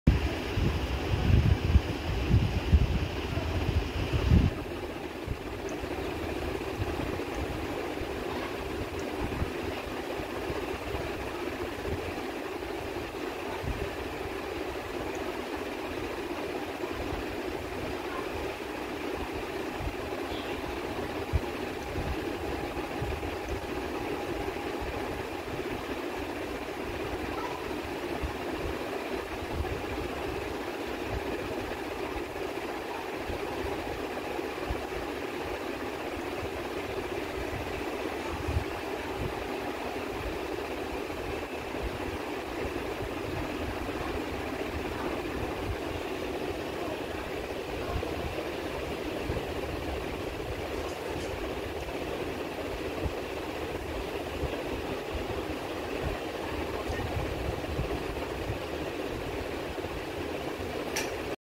Louis😼so thirsty#shorts#youtubeshorts
Louis is drinking water seems like he is so thirsty